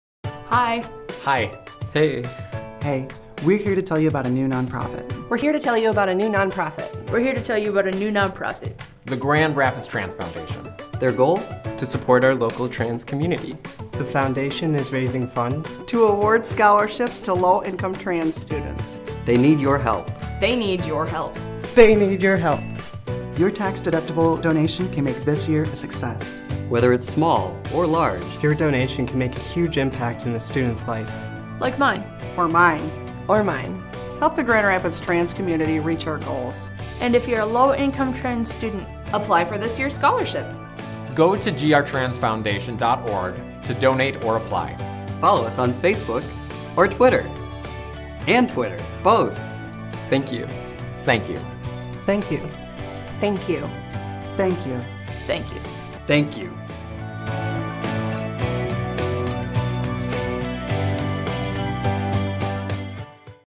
Grand Rapids Trans Foundation!
We're a nonprofit dedicated to creating opportunities for the trans community in Grand Rapids, MI!

If you're a trans student in Grand Rapids, MI, please apply for our scholarship!  If you're an ally, please consider supporting our organization!